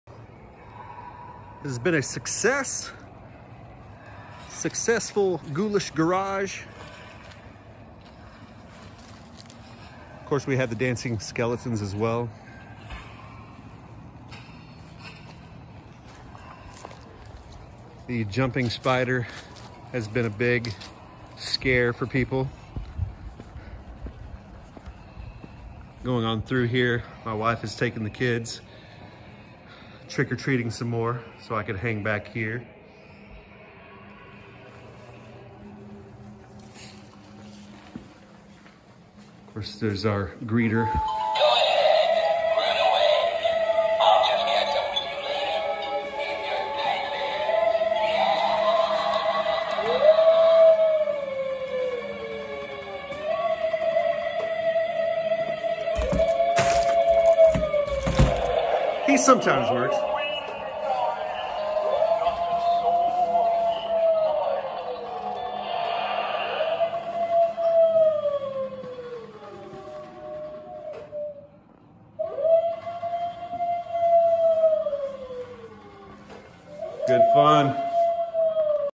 DIY Ghostly Garage 2 Haunted House Halloween 2021
Budget Haunted House
Projector lowes $100 atmosfx and more
bridal mesh to project on
homemade tombstones made by niece.
spirit halloween sale online for jumping spider $49
Clown animatronic $25 joyin product amazon
walmart hanging animatronics
Dollar general hanging light ups.
webs ordered on amazon.
Blacklights amazon

You can do a decent haunted house with inexpensive items.  Get to dollar tree or dollar general early for some decent extra items.